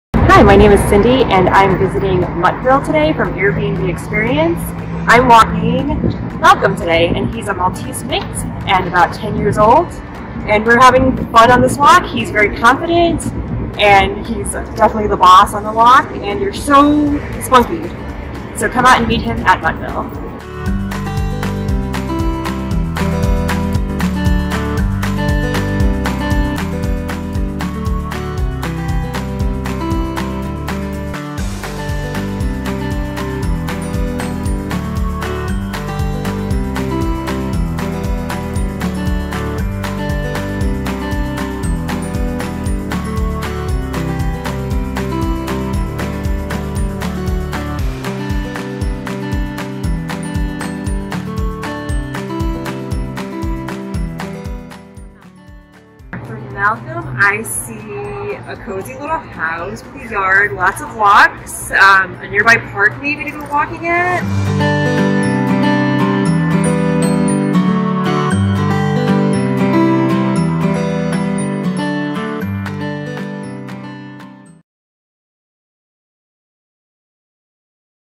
Malcom, a male Maltese Terrier mix-adopted!
Meet Malcolm, he had a great time with Cindy, who visited Muttville on the Airbnb Animal Experiences tour!
Special thanks to Robyn, Muttville volunteer videographer ♥